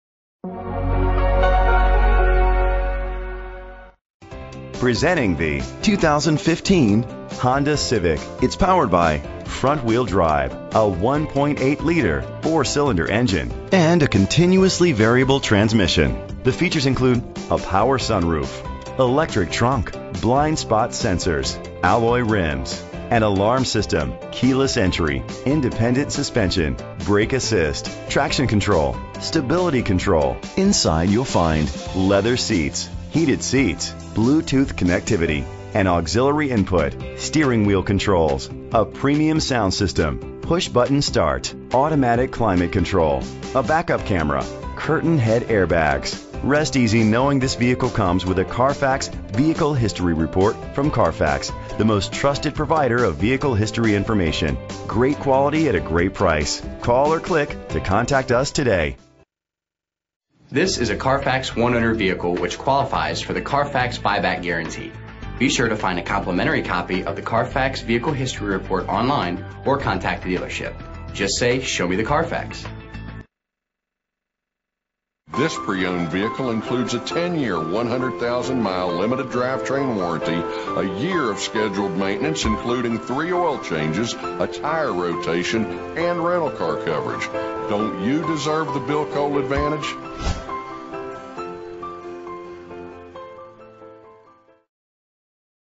2015 Honda Civic H3626A - Bluefield WV
Year: 2015
Make: Honda
Model: Civic
Trim: EX-L
Engine: 1.8 liter 4 Cylindercylinder FWD
Transmission: CVT
Color: White Orchid Pearl
Mileage: 37733
Address:
VIN:19XFB2F95FE031217